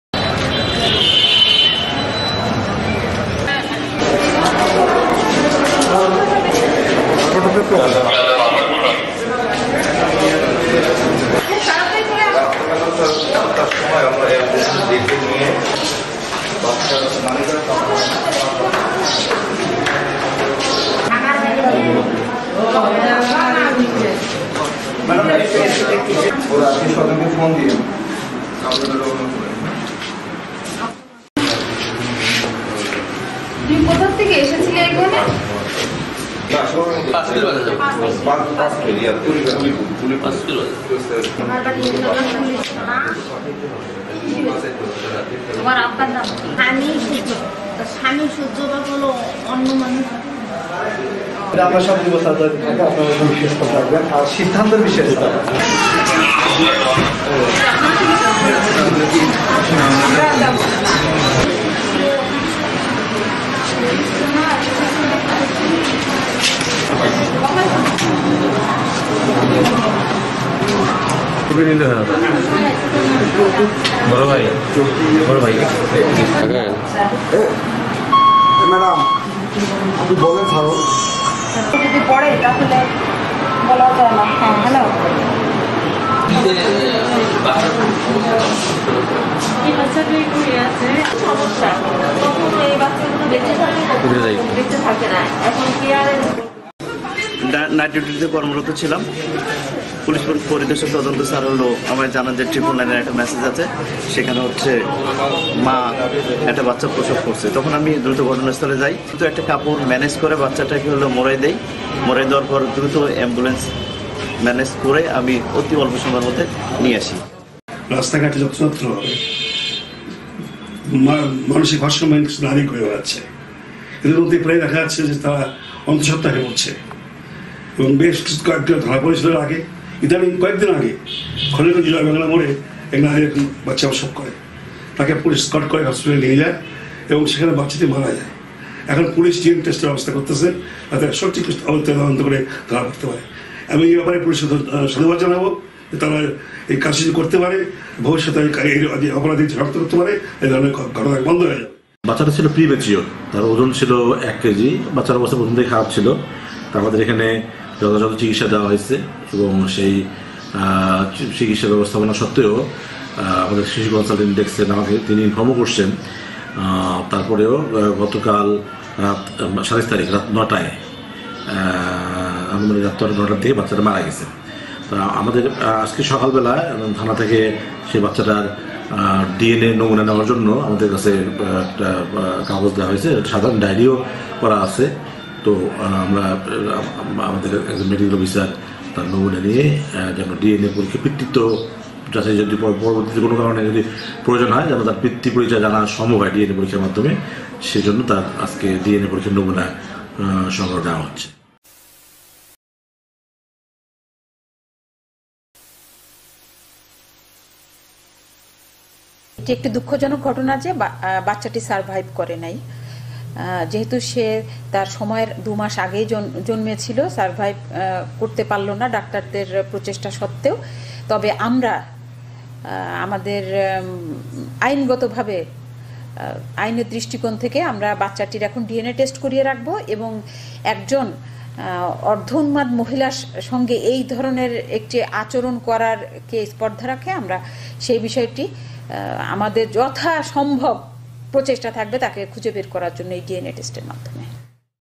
কুড়িগ্রামে অর্ধ উন্মাদ নারীর সন্তান প্রসব,ডিএনএ টেস্ট
কুড়িগ্রামে অর্ধ উন্মাদ নারীর সন্তান প্রসব।ডিএনএ টেস্টের মাধ্যমে অপরাধী সনাক্ত করা হবে।